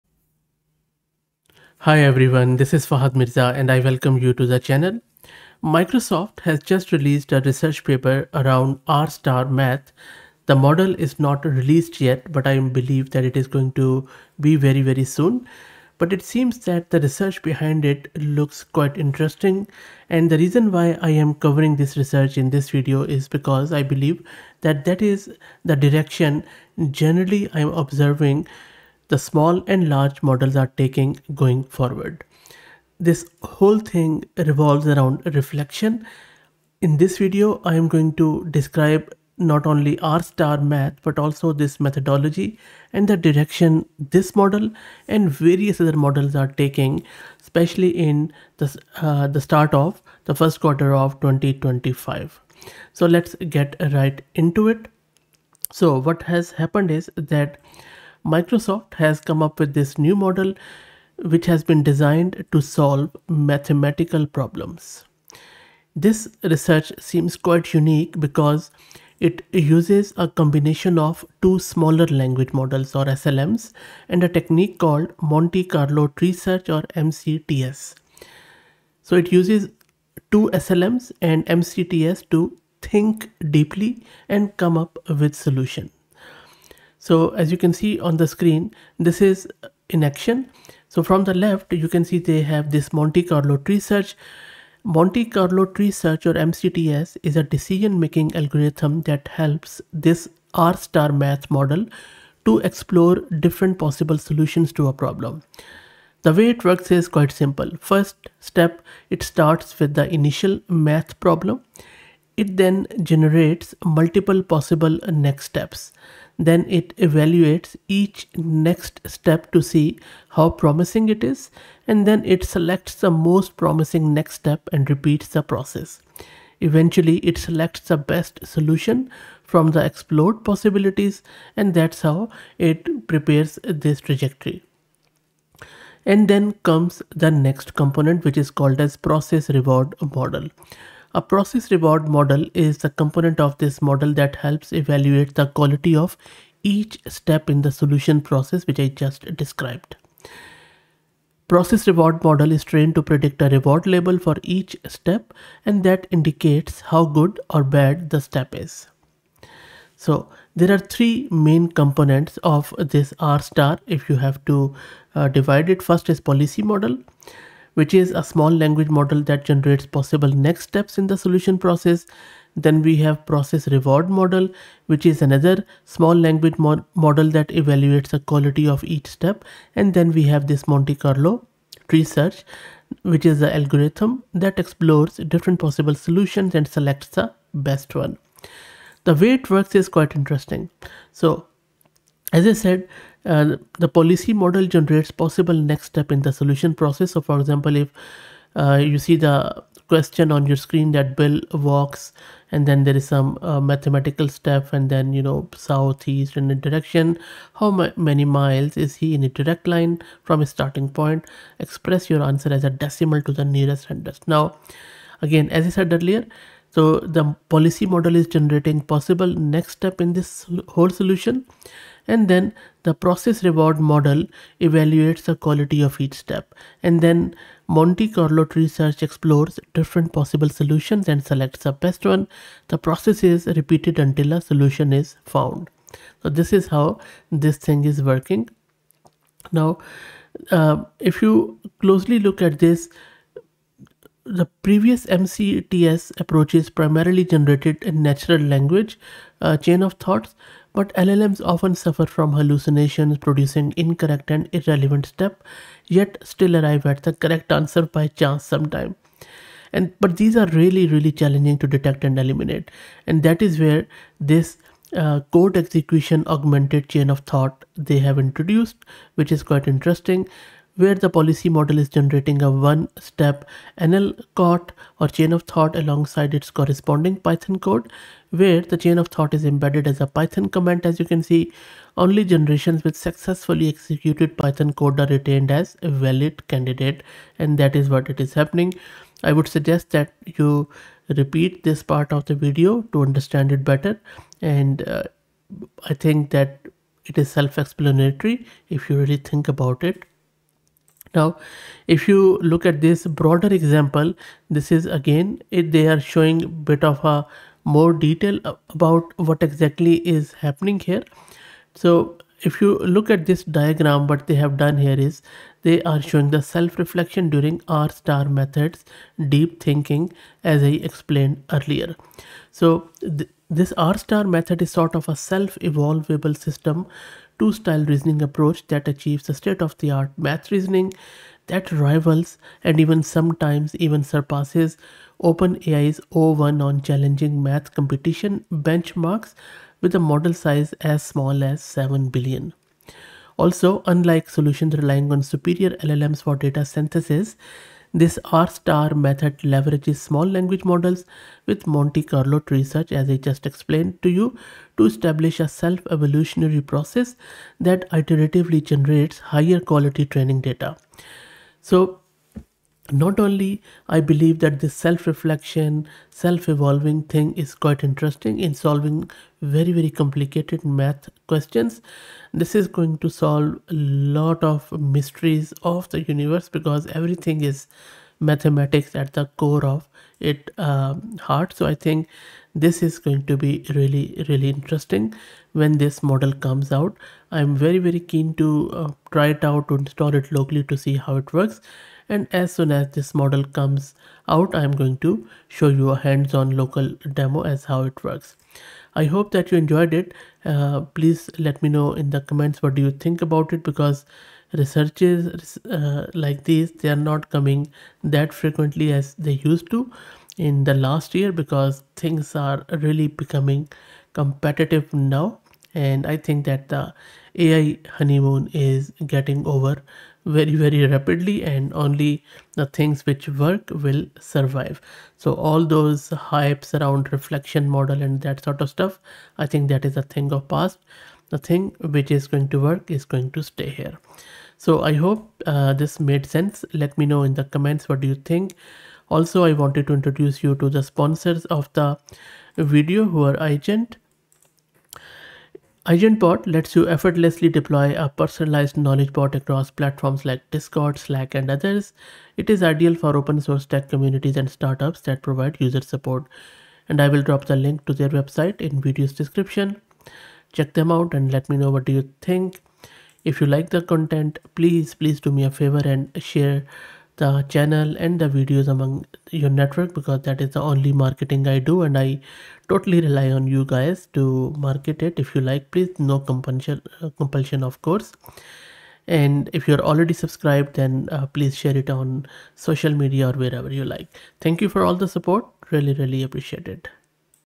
Microsoft rStar-Math: Self-Evolved Deep-Thinking Small Math AI Model
This video introduces rStar-Math which combines MCTS and Process Reward Model to increase inference-time compute, surpassing OpenAI o1-preview on MATH.